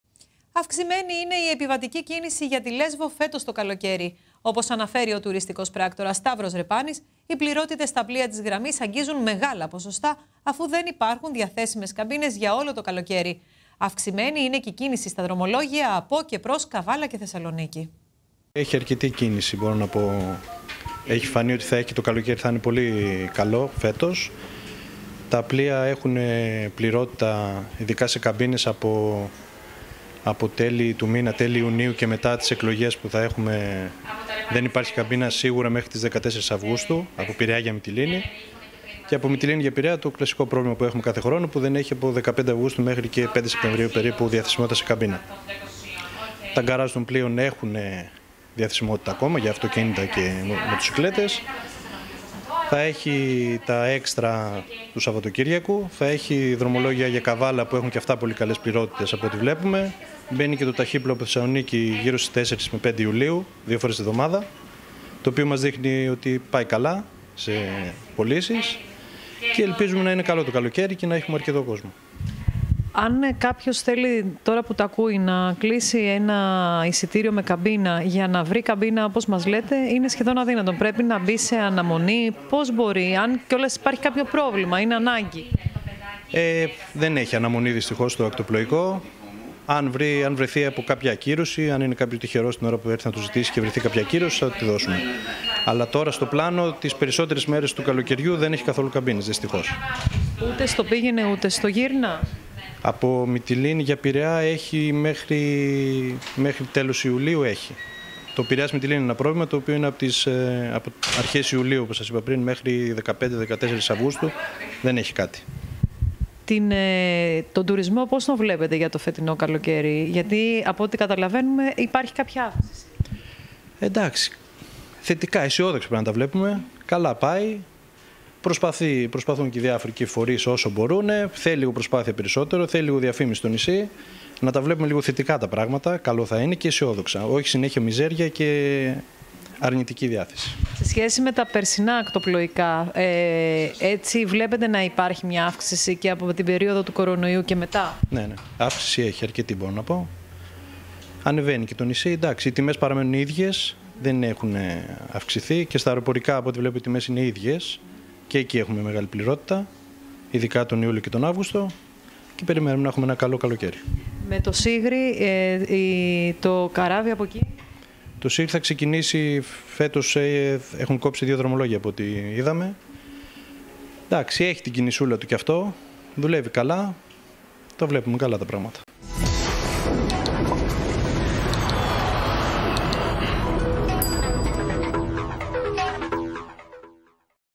Αυξημένη κίνηση στα πλοία το καλοκαίρι
Αυξημένη είναι η επιβατική κίνηση για τη Λεσβο φέτος το καλοκαίρι. Όπως αναφέρει ο τουριστικός πράκτορας Σταύρος Ρεπάνης οι πληρότητες στα πλοία της γραμμής αγγίζουν μεγάλα ποσοστά αφού δεν υπάρχουν διαθέσιμες καμπίνες για όλο το καλοκαίρι. Αυξημένη είναι η κίνηση και στα δρομολόγια από και προς Καβάλα και Θεσσαλονίκ